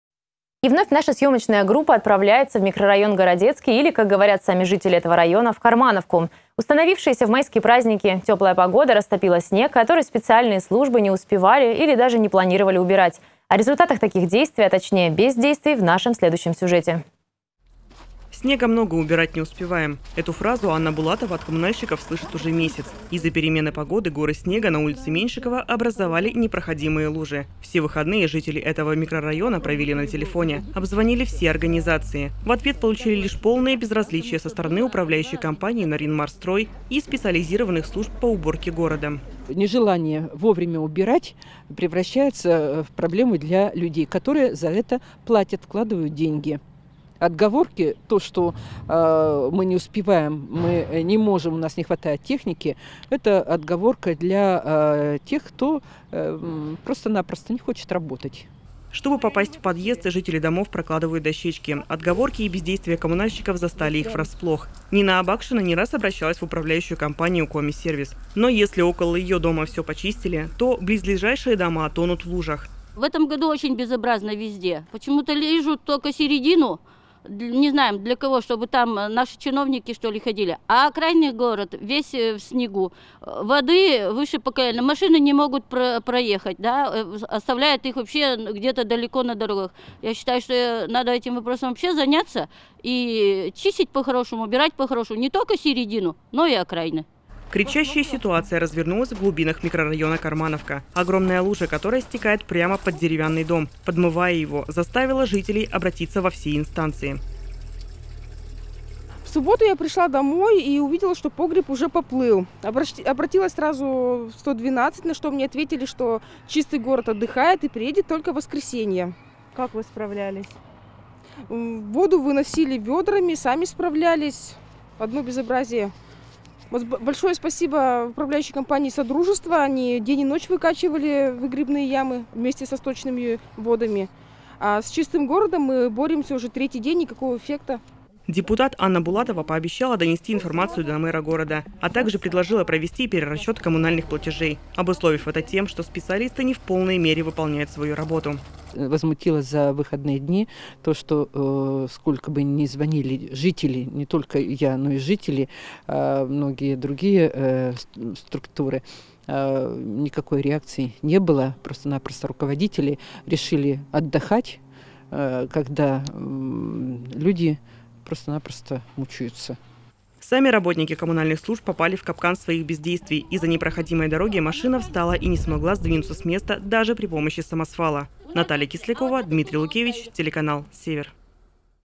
В микрорайоне Городецкий образовались непроходимые лужи
И вновь наша съёмочная группа отправляется в микрорайон Городецкий. Установившаяся в майские праздники тёплая погода растопила снег, который специальные службы не успели или даже не планировали убирать.